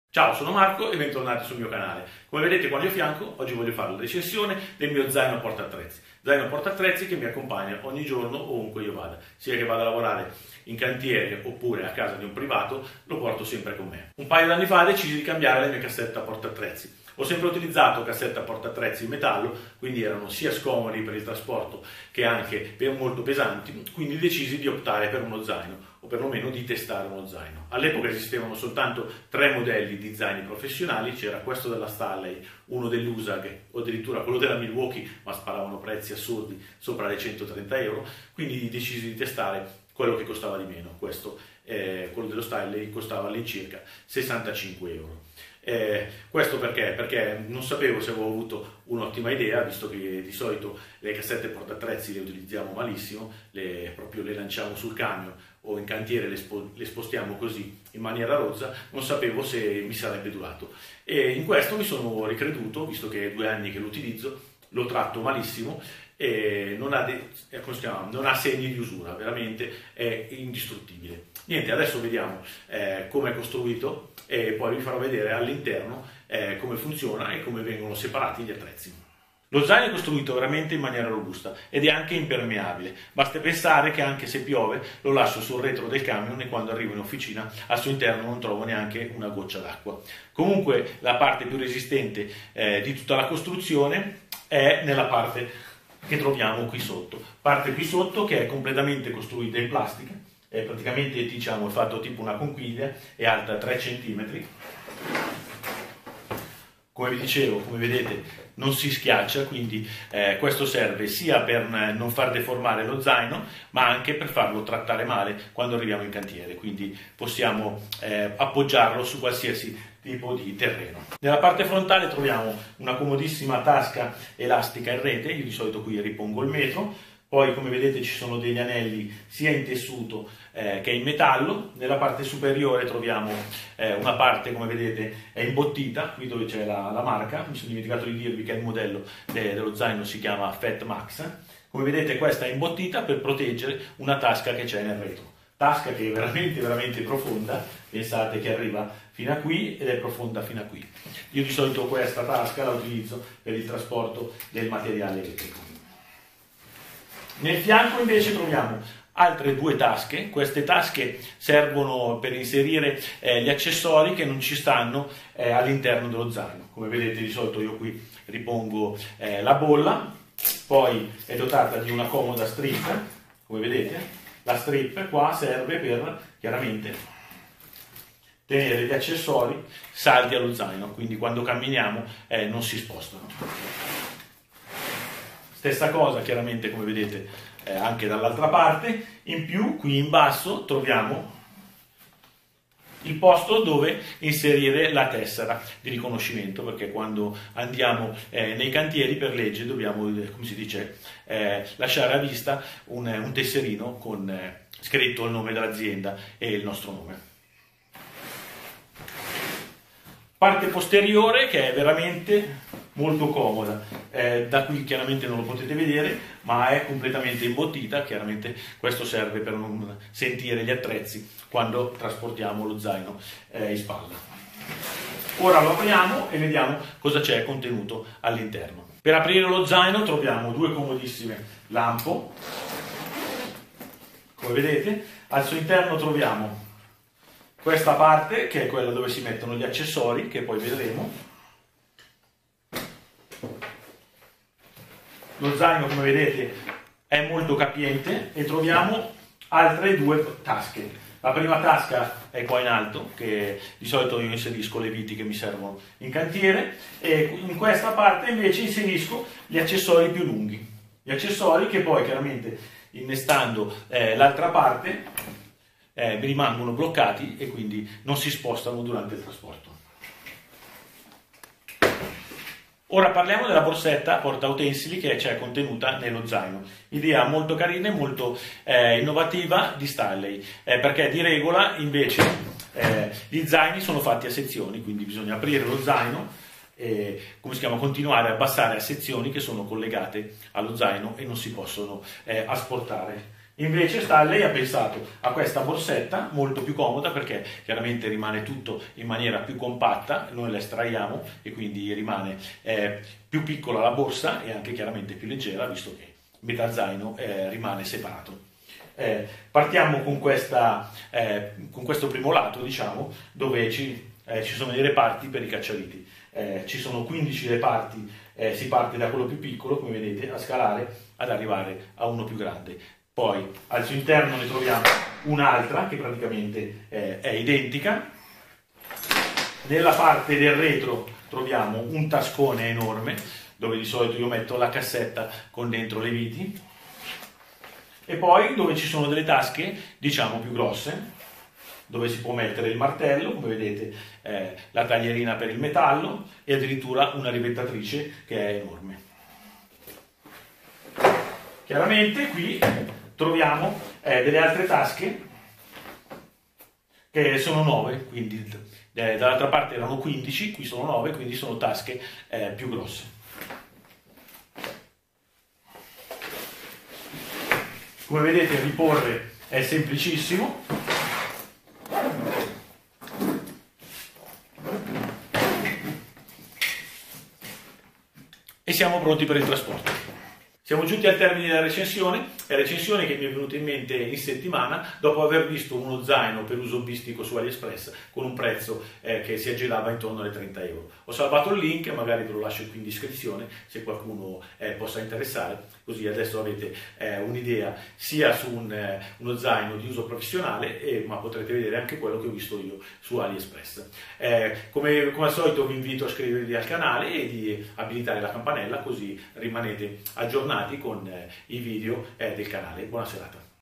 stanley fatmax zaino porta attrezzi
oggi voglio mostrarvi il mio zaino porta utensili, della stanley fatmax  zaino che posseggo oramai da diversi anni e che trovo veramente un accessori molto robusto e che mi sento di consigliare
LINK ZAINO amazon